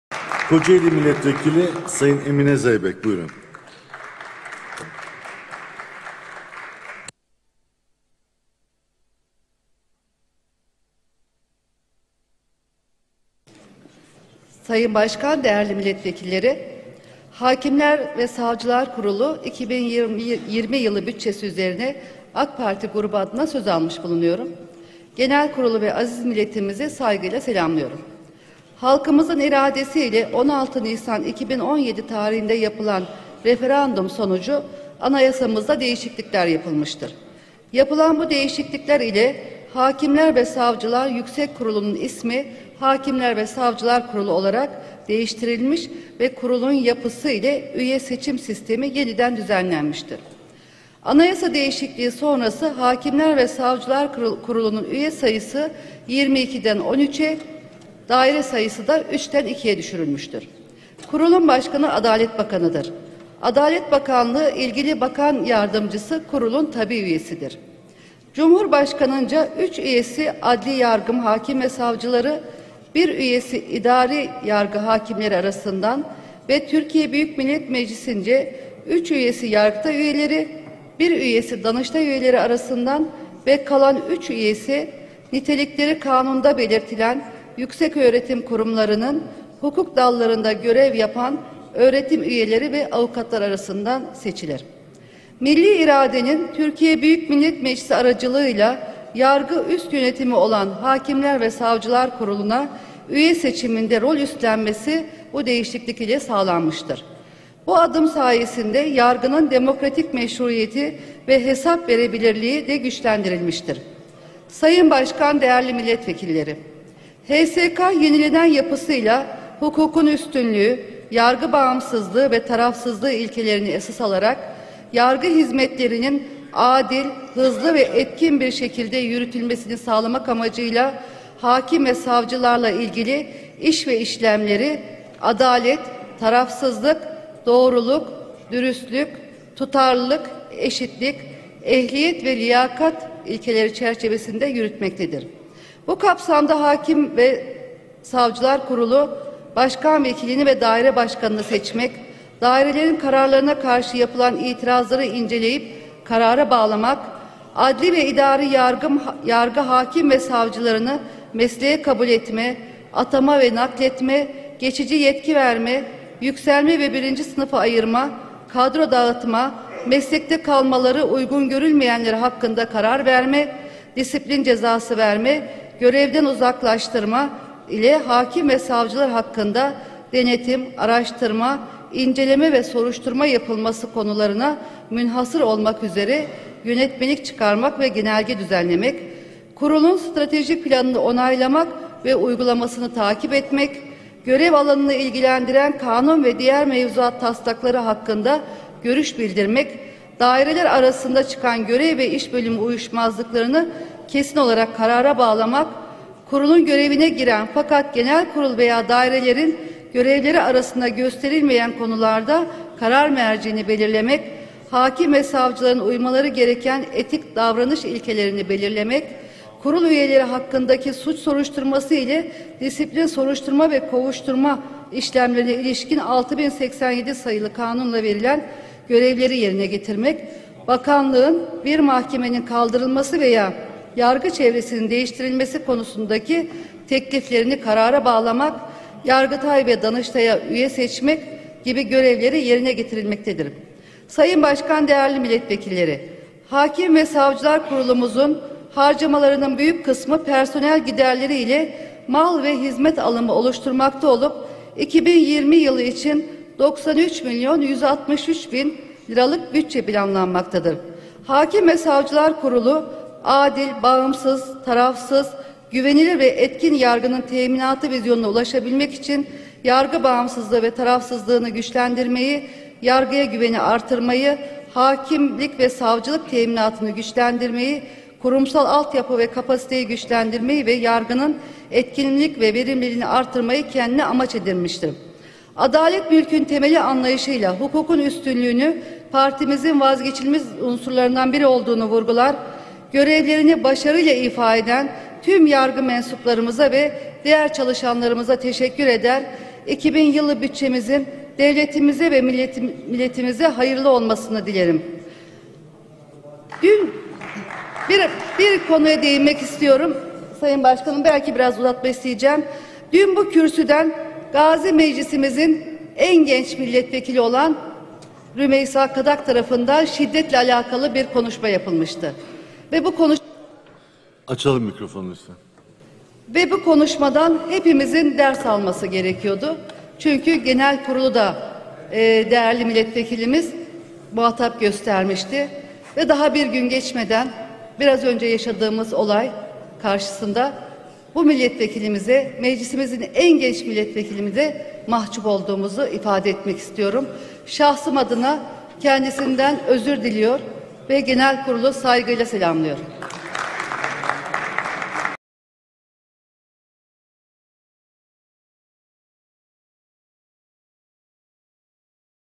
AK PARTİ KOCAELİ MİLLETVEKİLİ EMİNE ZEYBEK MECLİS KONUŞMASI-11 ARALIK 2019
ADALET BAKANLIĞI, YARGITAY, DANIŞTAY, MİLLİ SAVUNMA BAKANLIĞI, HSK, HAZİNE VE MALİYE BAKANLIĞI İLE BAĞLI KURULUŞLAR